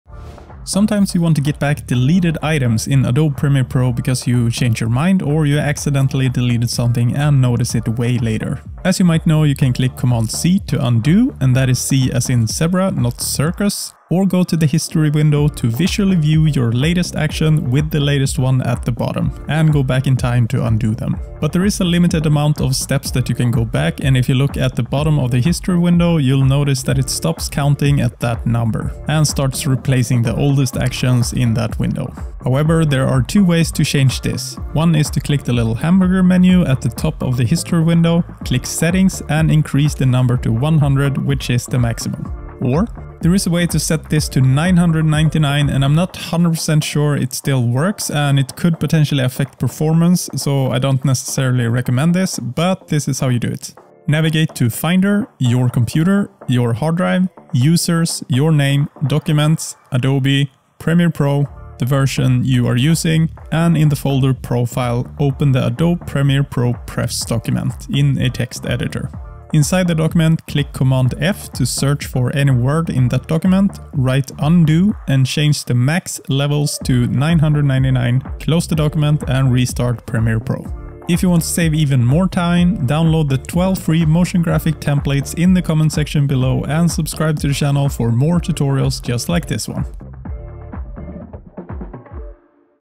How to get back deleted items in Premiere Pro
Sometimes you want to get back deleted items in Adobe Premiere Pro because you changed your mind or you accidentally deleted something and noticed it way later.

As you might know, you can click “cmd + Z” to undo (and that is Z as in Zebra, not Cirkus) or go to the “History” window to visually view your latest actions (with the latest one at the bottom) and “go back in time” to undo them

but there is a limited amount of steps that you can go back and if you look at the bottom of the “History” window, you’ll notice that it stops counting at that number and starts replacing the oldest actions in the window.

However, there are 2 ways to change this. One is to click the little hamburger menu at the top of the “History” window, click “Settings” and increase the number to 100 which is the maximum amount. Or?

There is a way to set this to 999 and I’m not 100% sure it still works and it could potentially affect performance so I don’t necessarily recommend this, but this is how you do it:
Navigate to Finder, your computer, your hard drive, Users, your name, Documents, Adobe, Premiere Pro, the version you are using, and in the folder Profile, open the “Adobe Premiere Pro Prefs” document in a text editor, click “cmd + F” to search for any word in that document, write “undo” and change “MaxLevels” to 999, close the document and restart Premiere Pro.